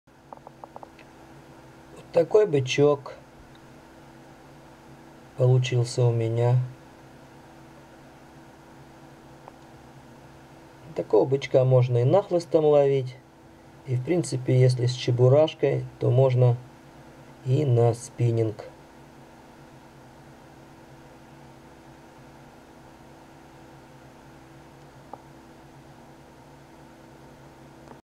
Бычок для хищника..
Не секрет, что в кормовую базу хищных рыб входит такая рыбка, как бычок подкаменщик. Его имитацию я и постарался связать.